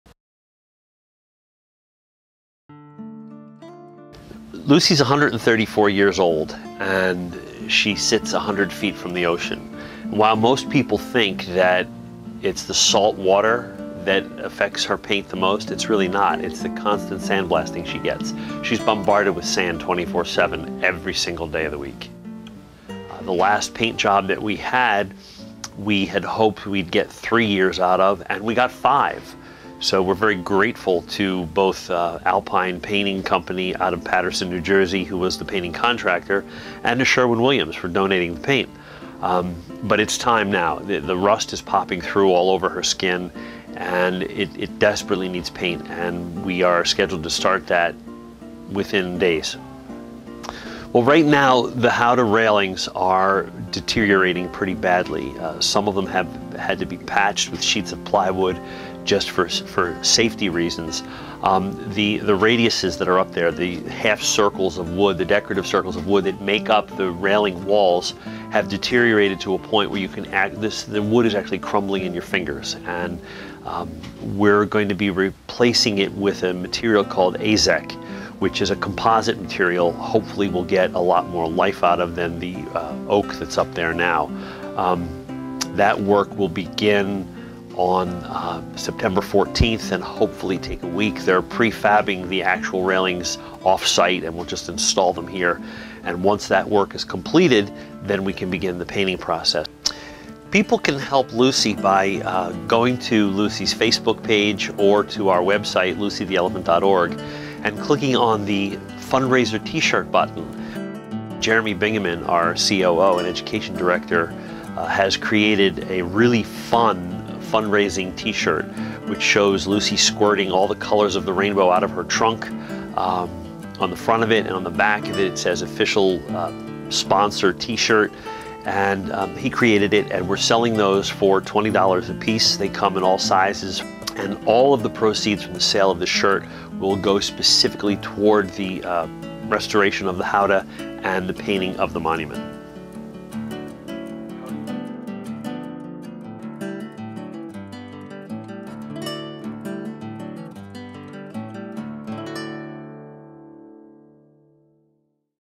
LUCY THE ELEPHANT-Needs Your Help
Lucy the Elephant , the 134 year old national Historic Landmark in Margate New Jersey needs your help. Lucy gets sandblasted every day and a new paint job is needed now! Please help preserve LUCY!!!